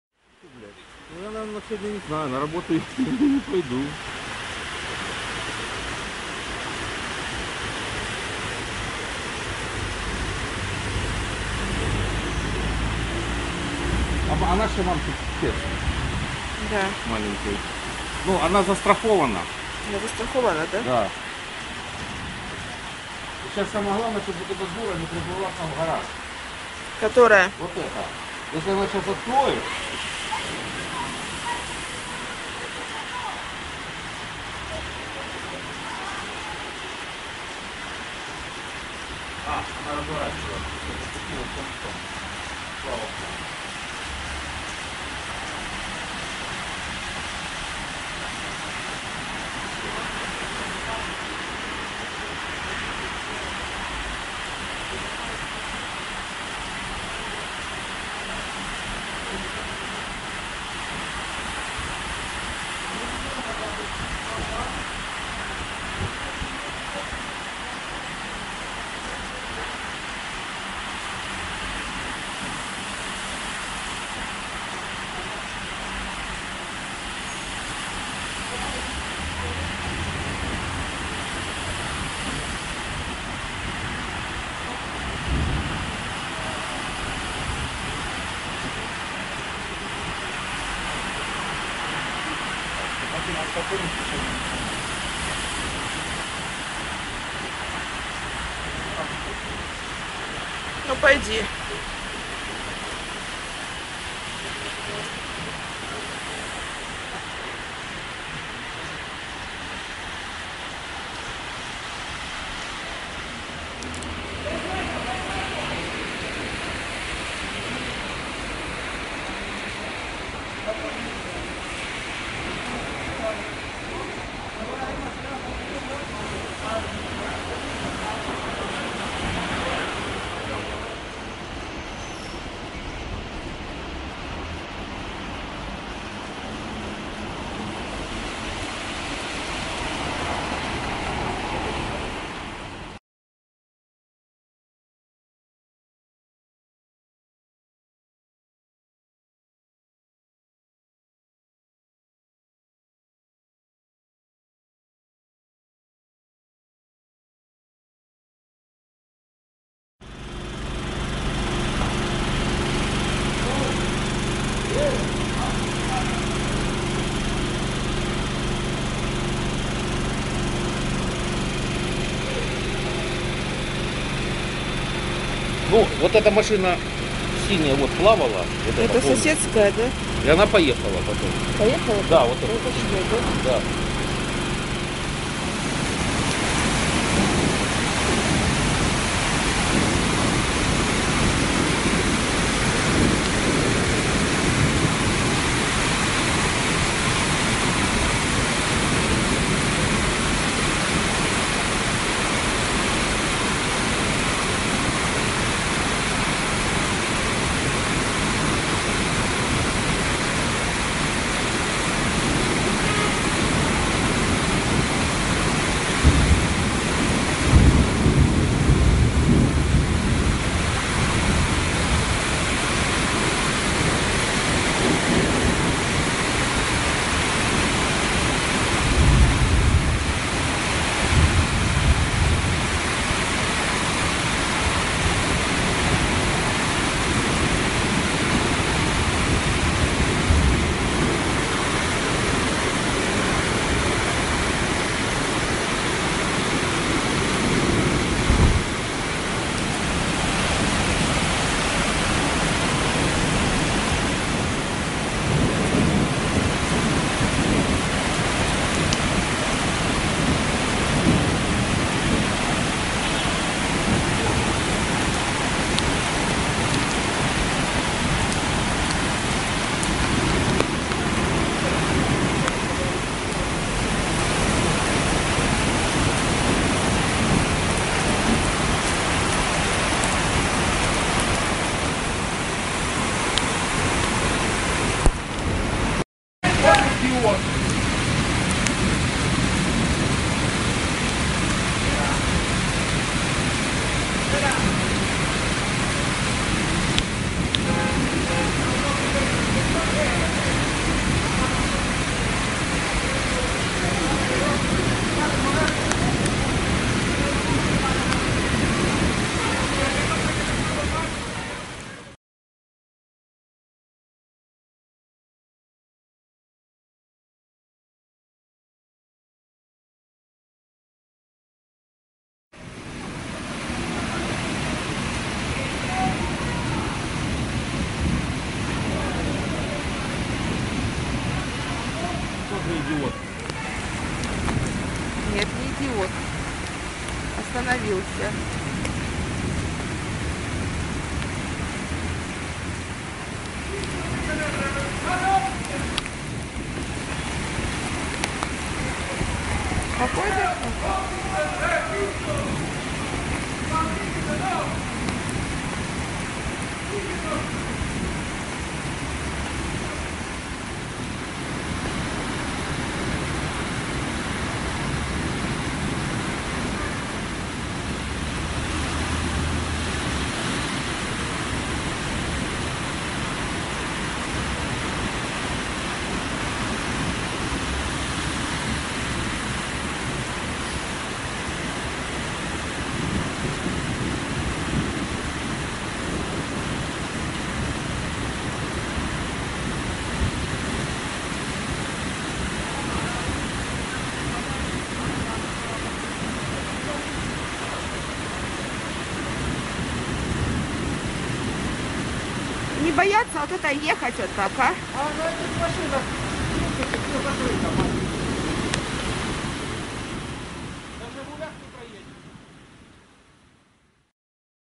Flooding Athens Alimos 2
February, 22nd 2013 Heavy rain wash out cars on Lysikratous 54, Alimos, Athens